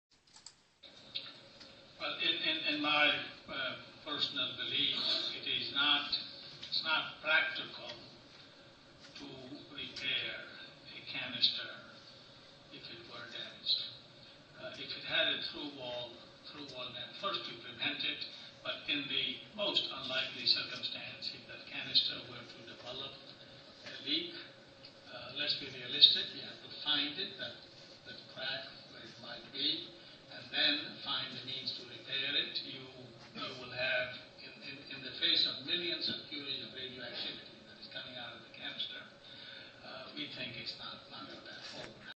Dr. Kris Singh - CEO of Holtec
"It is not practical to repair a cracked canister" (so the best solution is to prevent it from cracking by using a thick cask!)